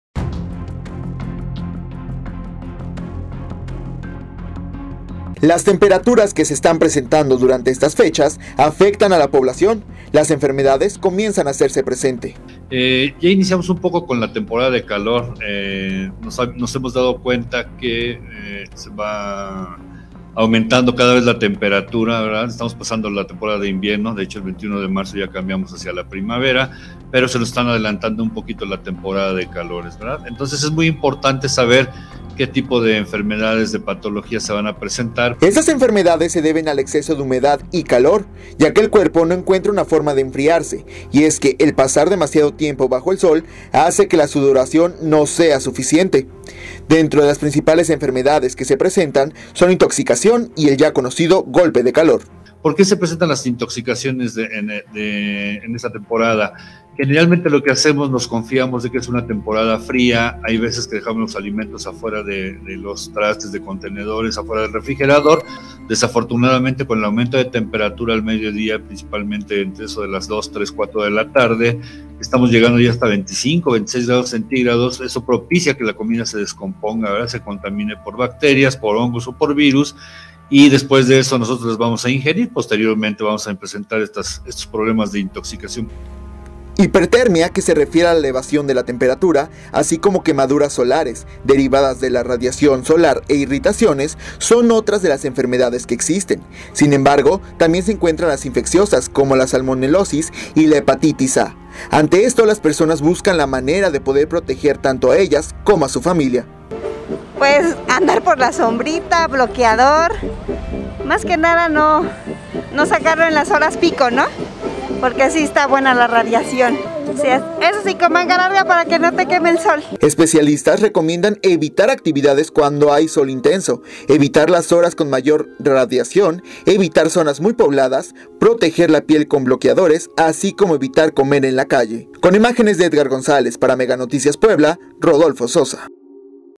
Especialistas recomiendan evitar actividades en temporada de calor
Principales enfermedades que se presentan son intoxicación y el ya conocido golpe de calor